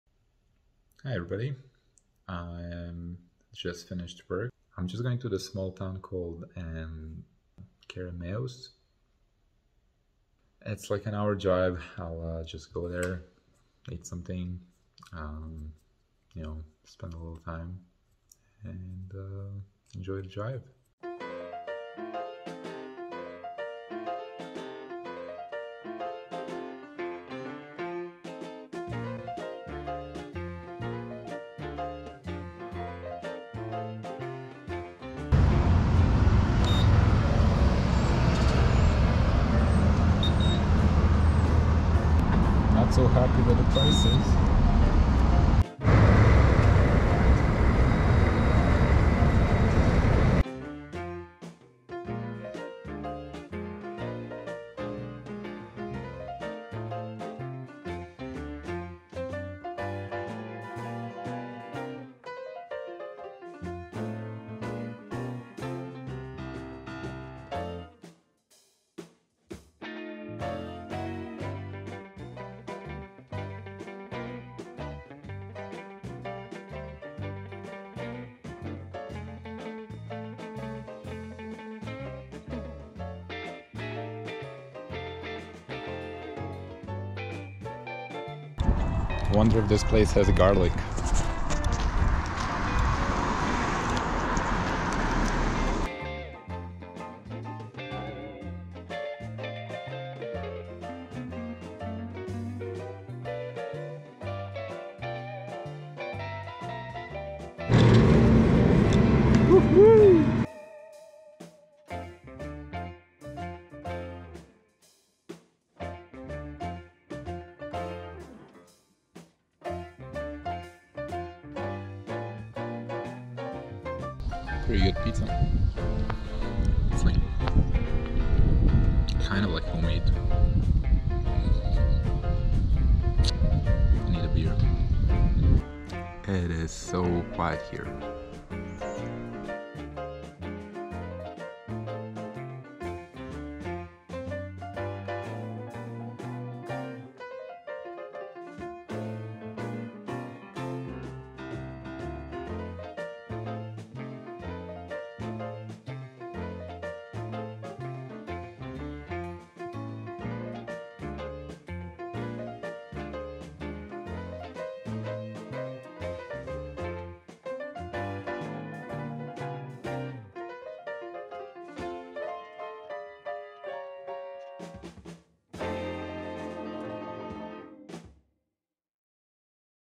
Keremeos, BC Road Trip
I spent the afternoon in Keremeos - the fruit capital of BC. Other than fruit, there is not much to do except sightseeing - parks, rivers, mountains. I tried a cheese pizza at Boboys pizza and it's definitely different and unique, as they make it deep dish style.